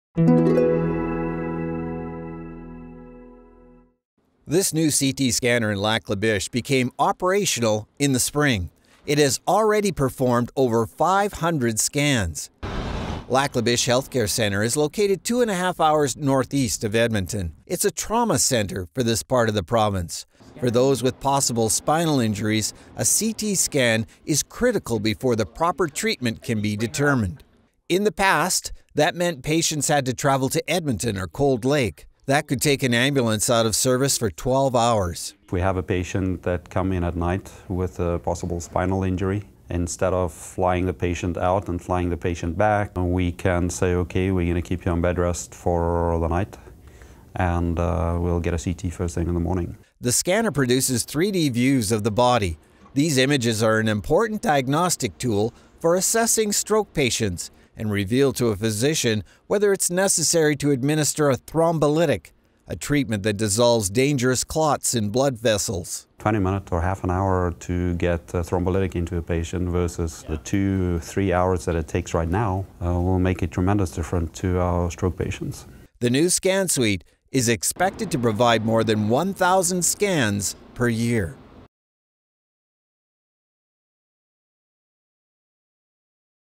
A new CT Scanner brings 3-D health care close to home for patients in the Lac La Biche area
The new CT scanner in Lac La Biche, which became operational in the spring, has already performed over 500 scans. The scanner marks a huge leap forward for patient care here, especially for patients suffering from possible spinal injuries or strokes.

Lac La Biche Healthcare Centre is located two-and-a-half hours northeast of Edmonton. It’s a trauma centre for this part of the province. For those injured in a traffic accident with signs of possible spinal injuries, a CT scan is critical in order to determine the proper treatment. In the past, that meant patients had to travel to Edmonton or Cold Lake.

The Lac La Biche Regional Health Foundation raised $2.1 million to fully fund the scanner. They were supported by large contributions from the Nashim family and Lac La Biche County.

The new scan suite is expected to provide more than 1,000 scans per year, serving patients from Lac La Biche and surrounding communities, including Buffalo Lake Metis Settlement, Hylo, Kikino, and Plamondon.